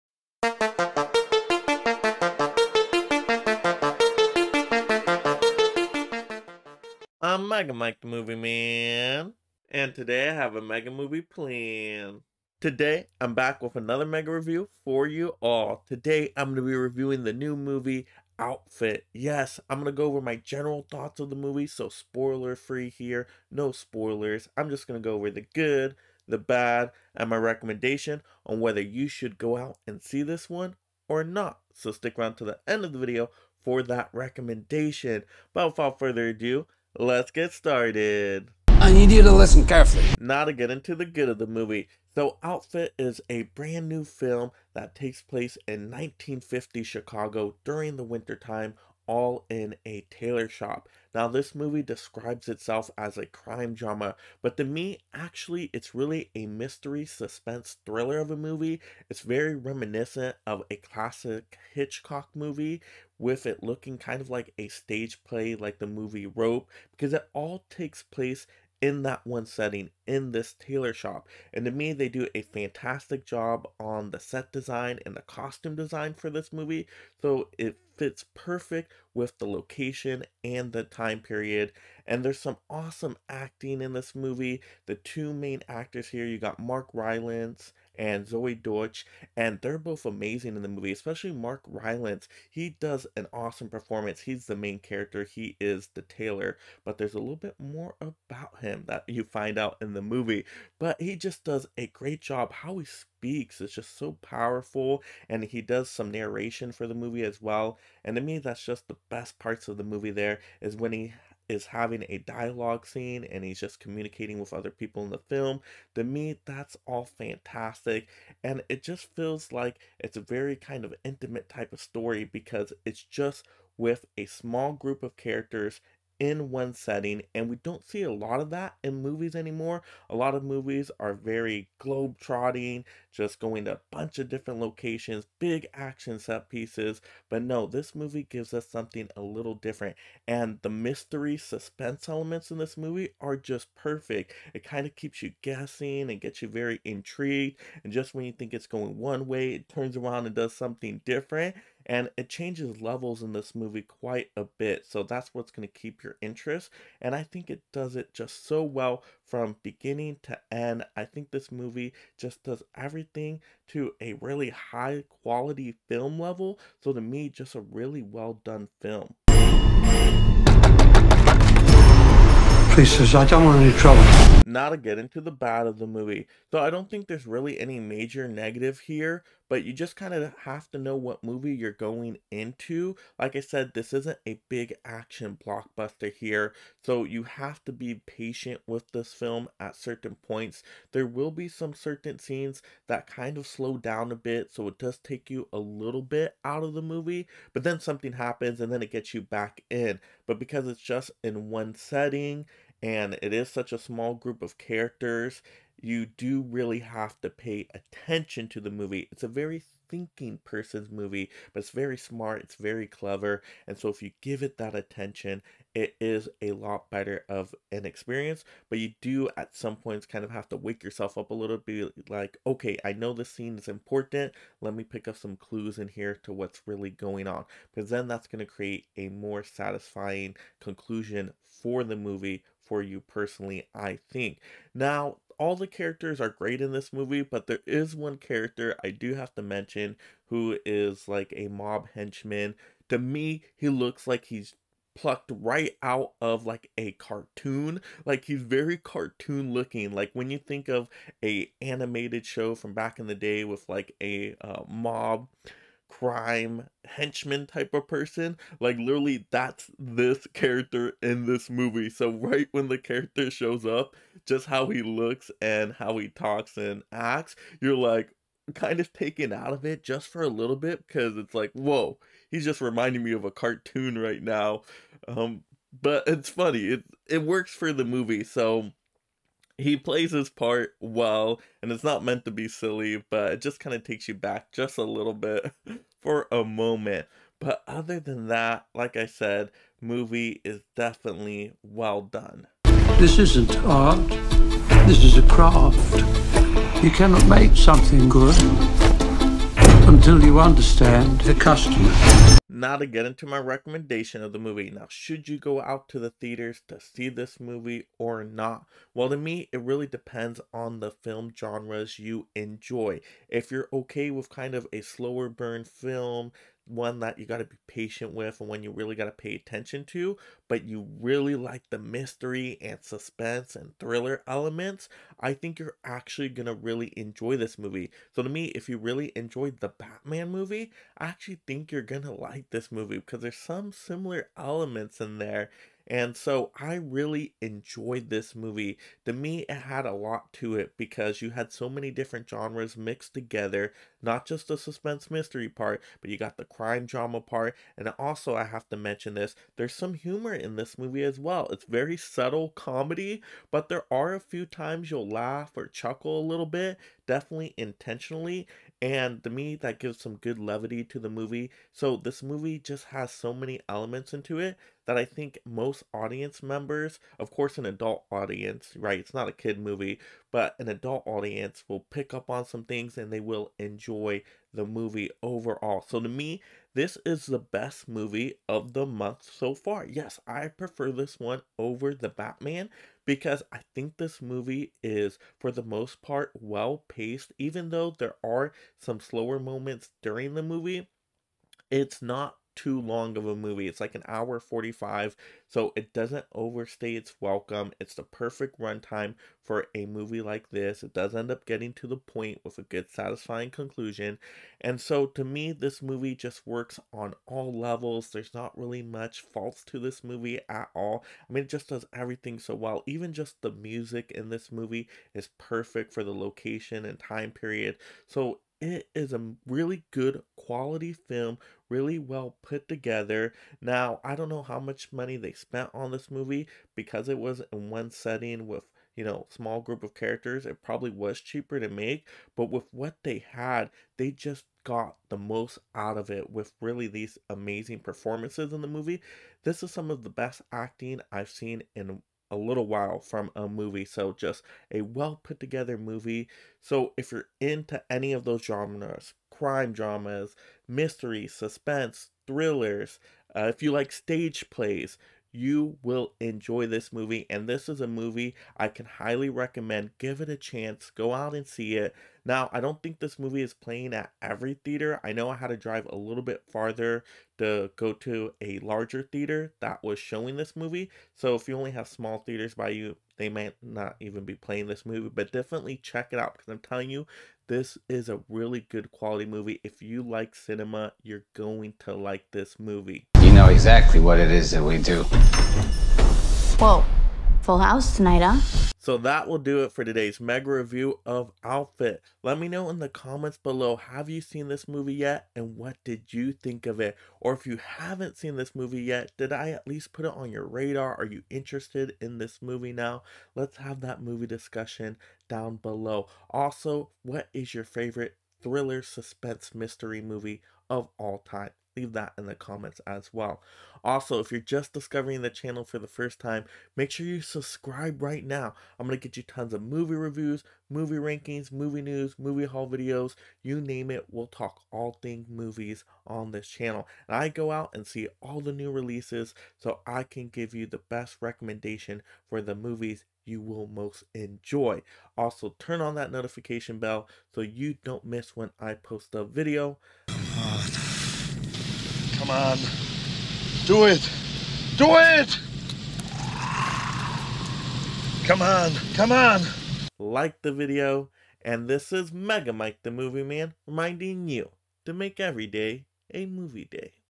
The Outfit MOVIE REVIEW - Better than The Batman!
This a review (spoiler free) on the new movie The Outfit (2022). I go over my general thoughts of the movie: the good, the bad and my recommendation on whether it is worth your time or not. So stick around till the end of the video for that recommendation. Also, leave in the comments below if you have seen this movie and your thoughts of it? Or if you're planning on seeing this movie now?